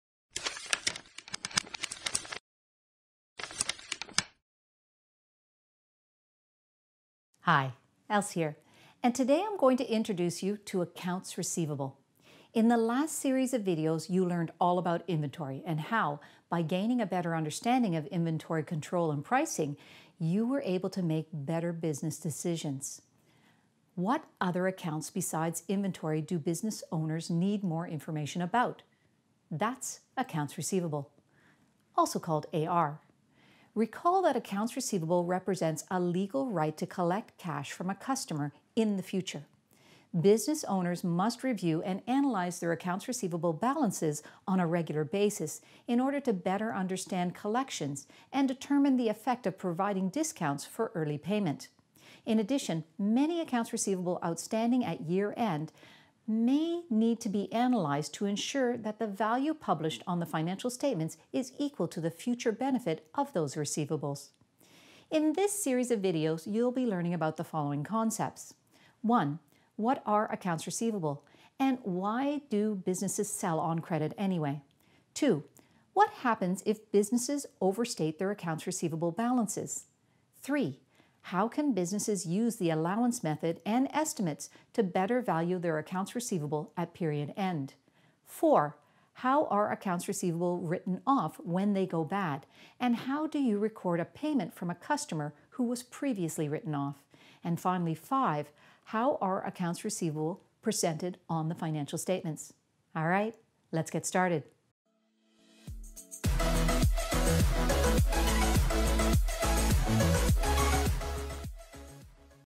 Chapter 7, Accounts Receivable, Introduction
This video introduces the concepts in Chapter 7 of ACC100, Introductory Financial Accounting, at Ryerson University.

Note that this course does not include the use of debits and credits but instead uses the accounting equation to analyze business transactions.

Other students may find these videos valuable for their learning.

Else